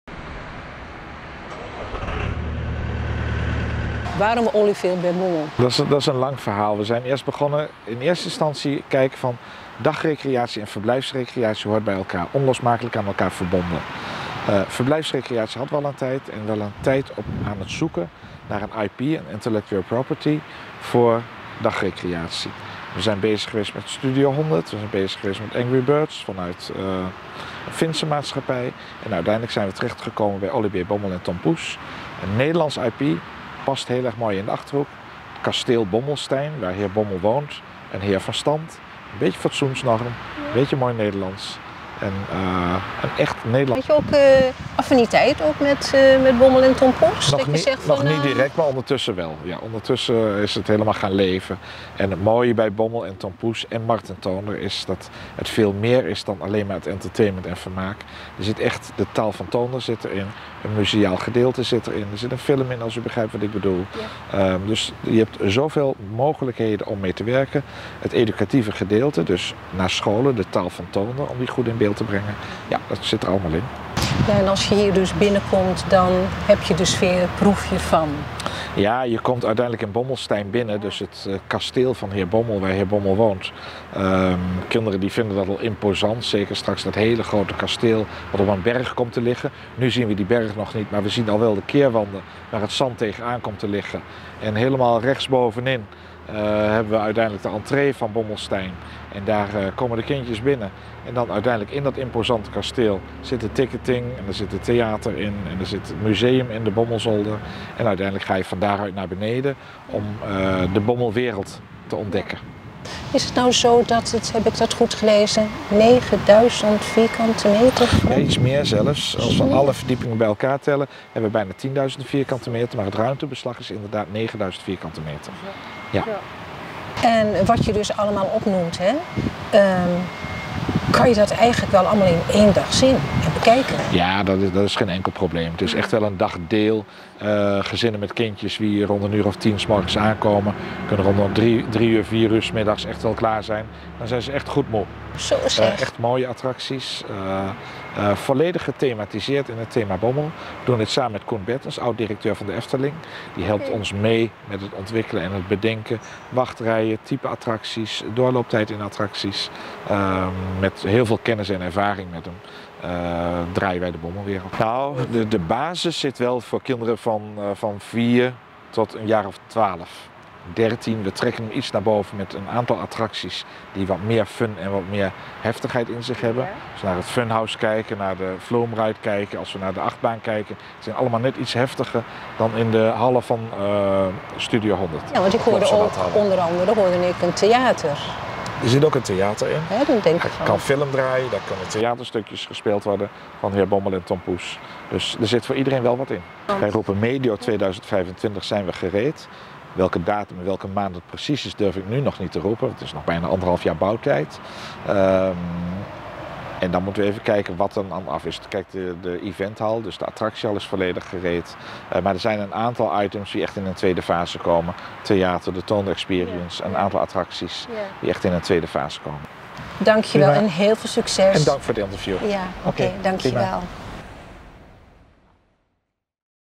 20240708 Haaksbergen Nieuwbouw Attractiepark Bommelwereld
VIDEO Nieuw atractiepark brengt de wereld van Bommel tot leven

De bouw van een nieuw indoor themapark 'Bommelwereld' in de Achterhoek is van start gegaan.

Bommelwereld zal een onderdeel van het bestaande Gelderse vakantiepark Marveld Recreatie gaan worden. Het park draait om de stripfiguren Tom Poes en Olivier B. Bommel.

Het themapark krijgt een oppervlakte van 9000 m2 en wordt ontwikkeld door Marveld recreatie in samenwerking met de Toonder Company. De karakters van Tom Poes en Heer Bommel staan centraal in de gethematiseerde kasteelomgeving.

Edwin Bomers van Marveld Recreatie geeft aan dat niet alleen gedacht is aan de binnenkant, hij voorspelt dat het exterieur van de attractiehal een stuk fraaier wordt dan vergelijkbare loodsen bij andere parken als Plopsa Indoor en Toverland.

De opening van Bommelwereld zal plaats vinden in het voorjaar van 2025.

Kijk voor meer informatie op de website van Bommelwereld

Camera en montage: Matthijs Prins
Interview: Hannah Hilgen
Eindredactie: Michel van der Voort

RTV Sternet is een onafhankelijk journalistiek nieuwsmedium voor Haaksbergen.